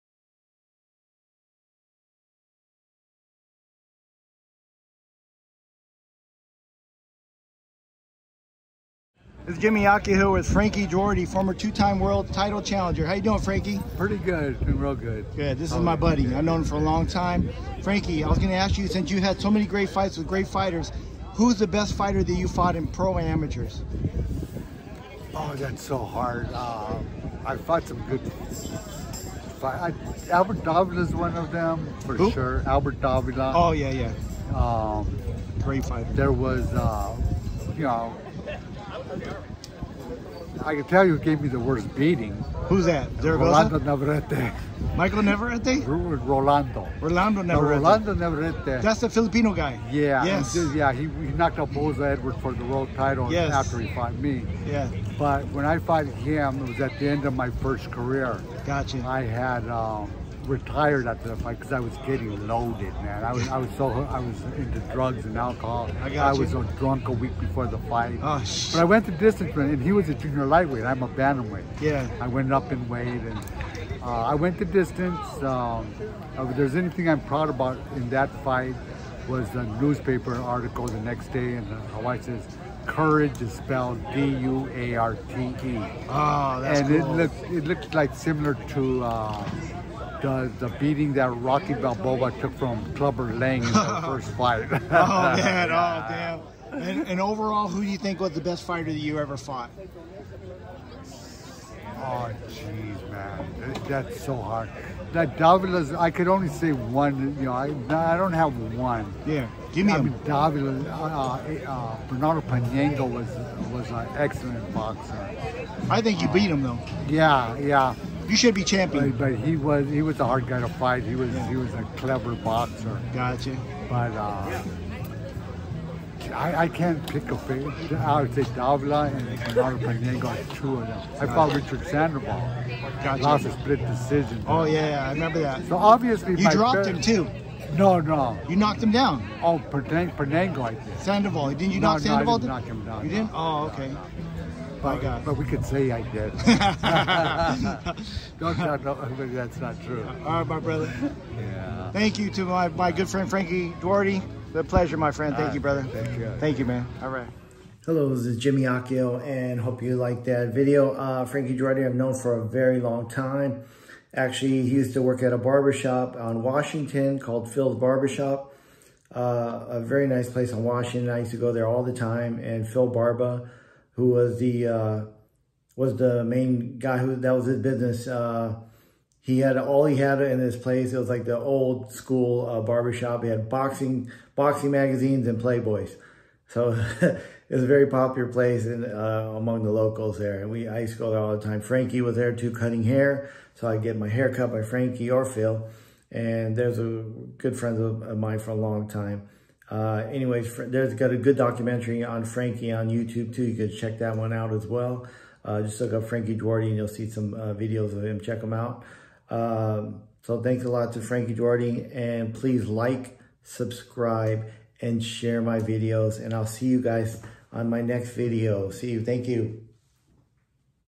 Frankie duarte on who gave him his worst beating n best fighter he fought
Frankie duarte talks about the best fighter has has fought n who gave him his worst beating. Very funny interview.